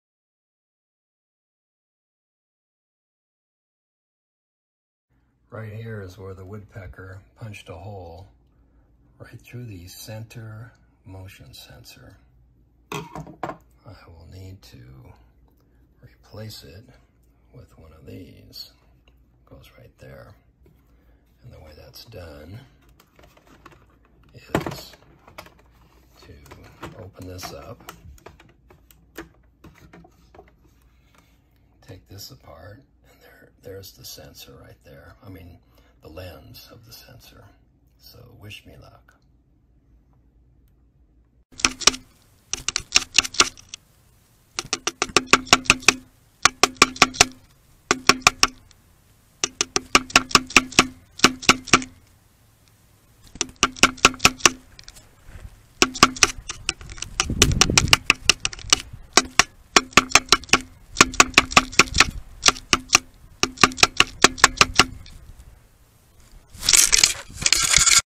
Trail Camera Damaged by Woodpecker
A Gila Woodpecker stopped by to peck the daylights out of my trail camera. See the woodpecker in action at the end of this video. The cam lens is hard plastic so no damage there. But it pecked holes right through the center motion sensor Fresnel lens, which is thin plastic.